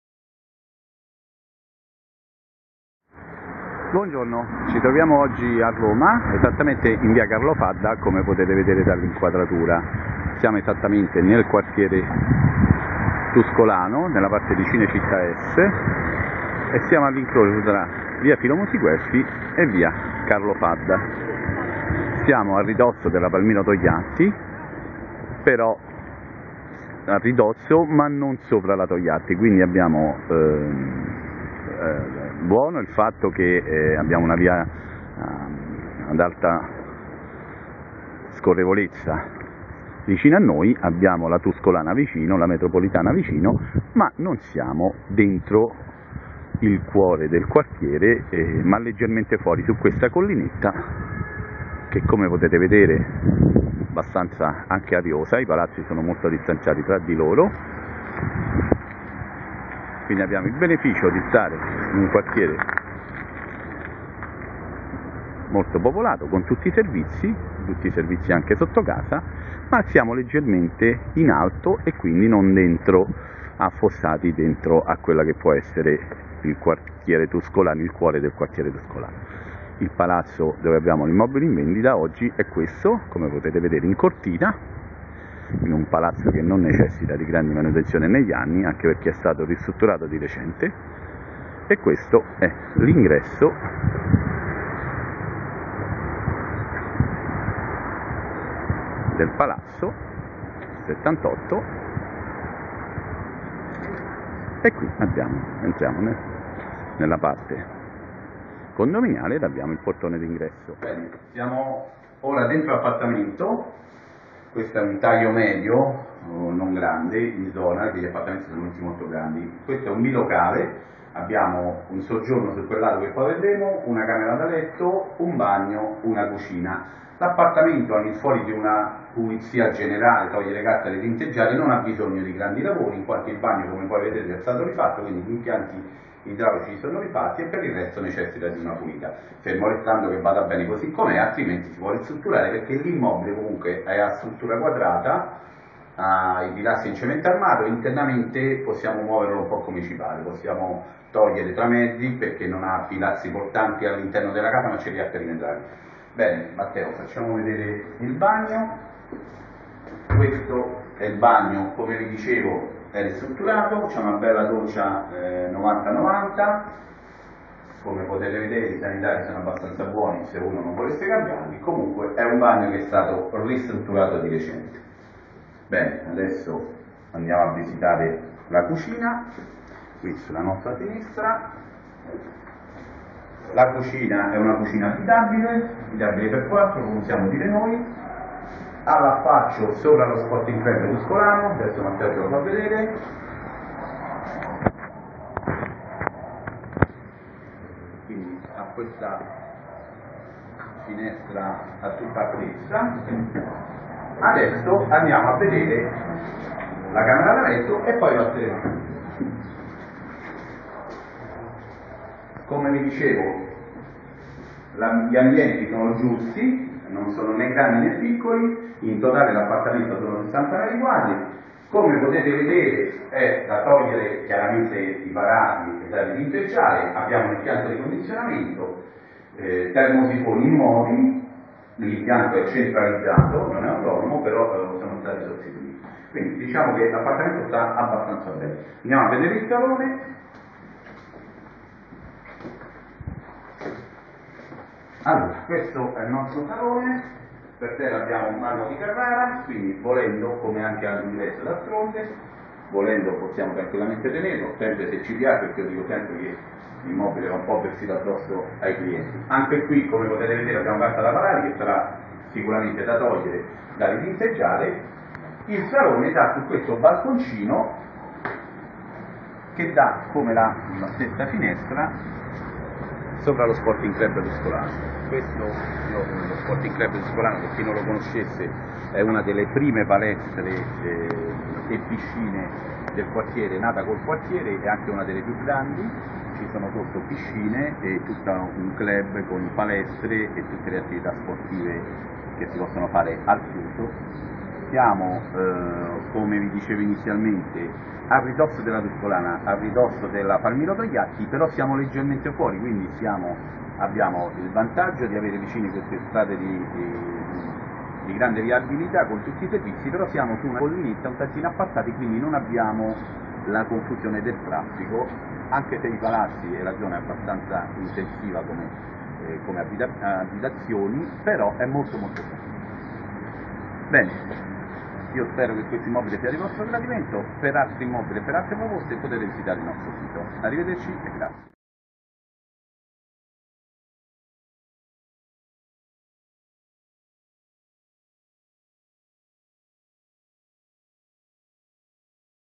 VIA CARLO FADDA 78
Appartamento via Carlo Fadda di 60 mq posto al 4° piano composto da: ingresso, salone, cucina abitabile, camera matrimoniale, bagno recentemente ristrutturato. Balcone. Ascensore. Stato dell'immobile abitabile.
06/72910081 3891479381